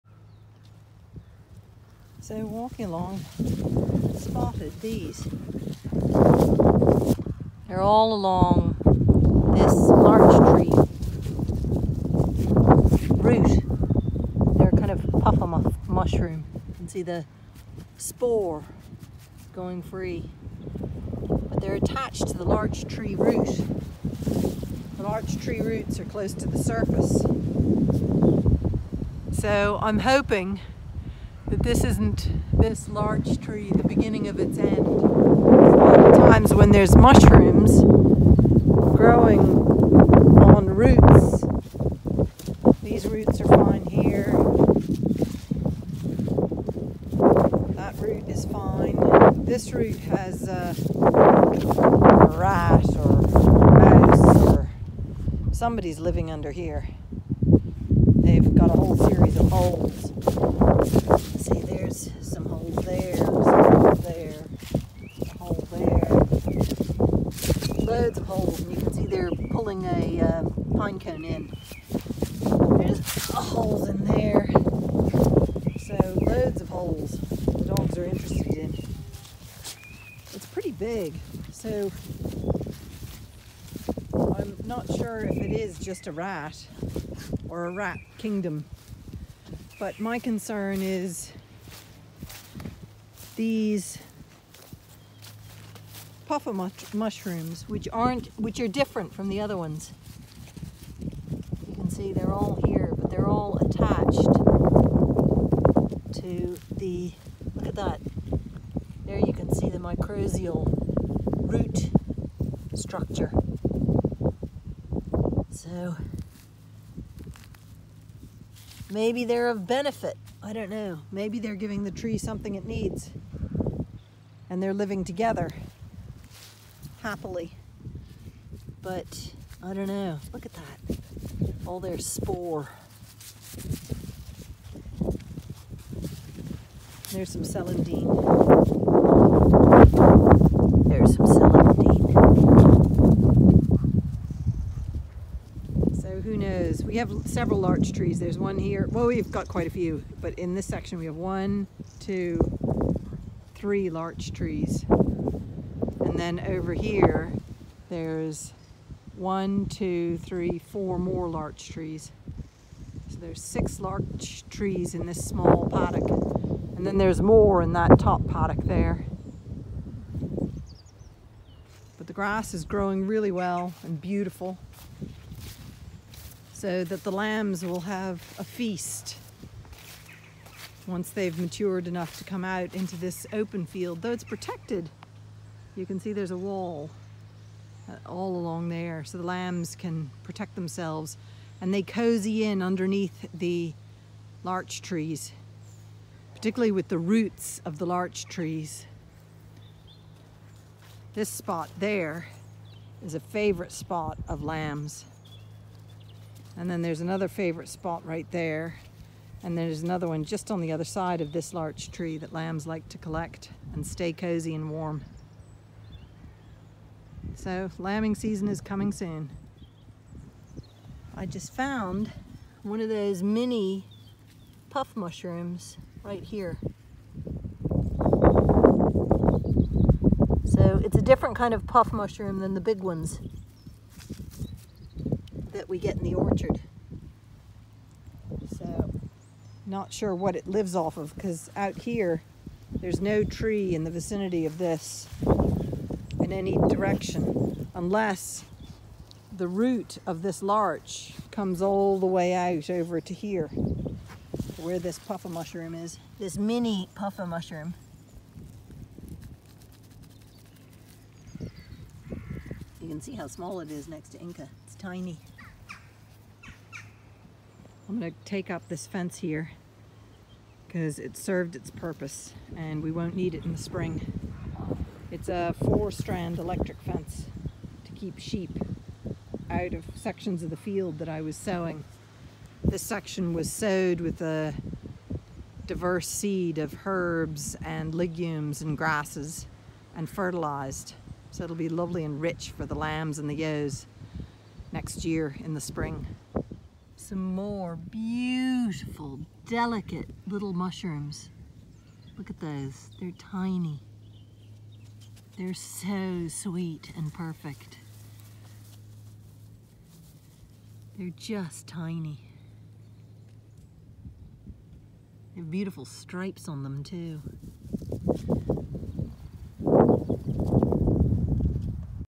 Some interesting minny puffball mushrooms on a larch tree root
What love about farming with nature is always discovering new things. Today it’s these, new to me, is this colony of small puff ball mushrooms.